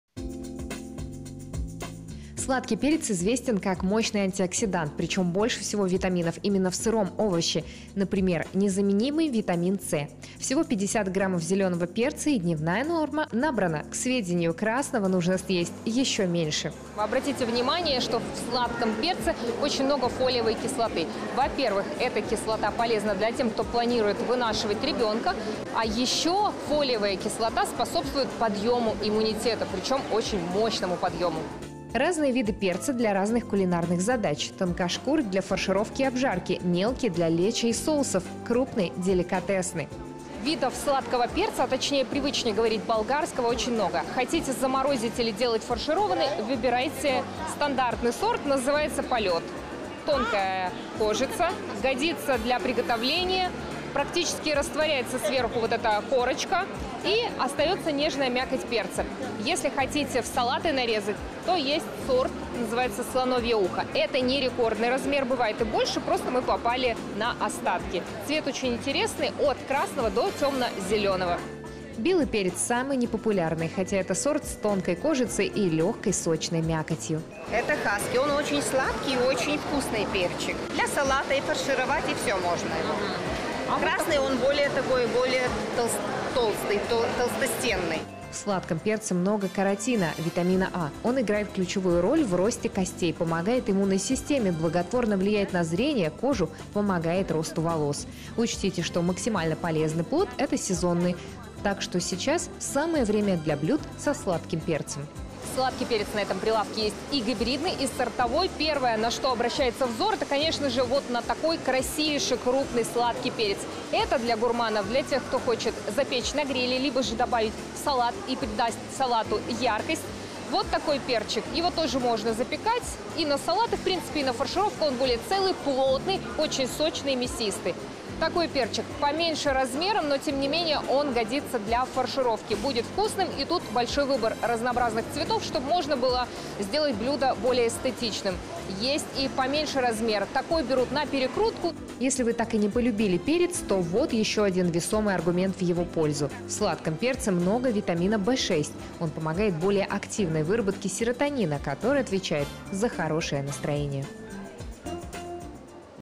Чем же так хорош болгарский перец? Следим за базаром
Чем же так хорош болгарский перец? Следим за базаром. На рынке сезон сладкого перца. Как этот овощ может заменить целую миску разных овощей. Изучаем пользу сладкого перца и берём на заметку.